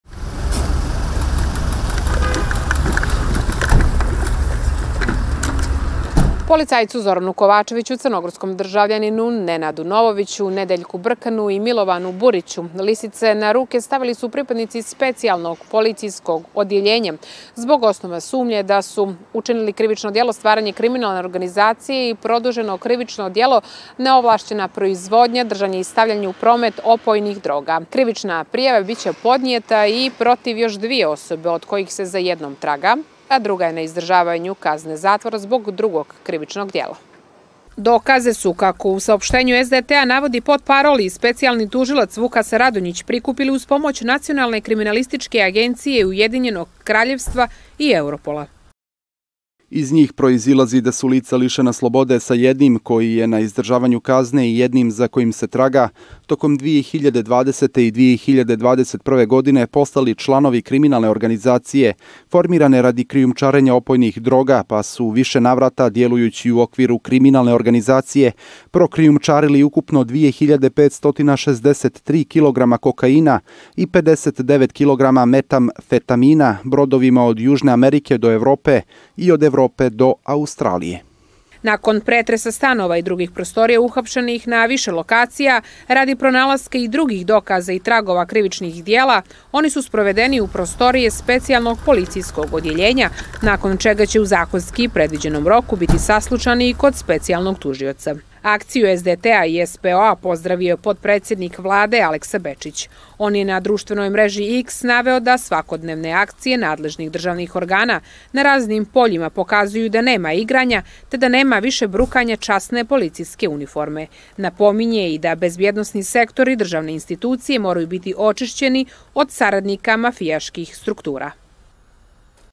NOVA - ŠVERCOVALI OD AMERIKE DO AUSTRALIJE, IRENA RADULOVIĆ 25.12.2023.
Zbog sumnje da su učestvovali u krijumčarenju više od 2,5 tone kokaina i 59 kilograma metamfetamina jutros su po nalogu Specijalnog državnog tužilaštva uhapšene cetiri osobe medju kojima je i sluzbenik Uprave policije, uhapšen u stanici granične policije u Baru. Oni su, kako se navodi u saopstenju SDT-a tokom 2020. i 2021. godine, postali članovi kriminalne organizacije formirane radi krijumčarenja opojnih droga.